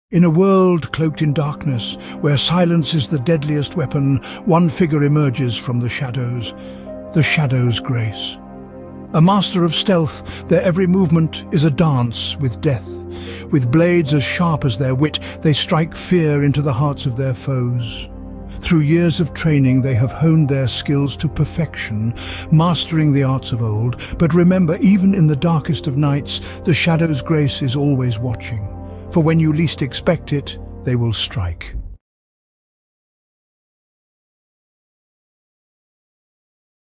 Shadow's Grace: Dance with Death
Here we have a Video that explains to us the life of the Ninjas.

made in InVideo.ai